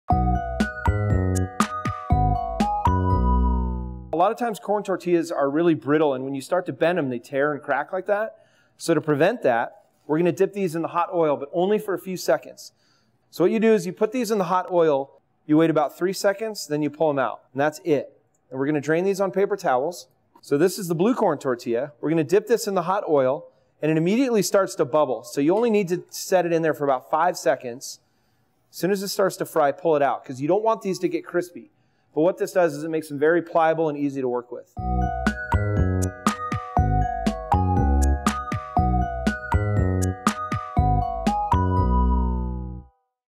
Tillamook Kitchen Quick Tips: How To Prevent Tortillas From Breaking
Learn how to properly cook corn tortillas without having them crack or break.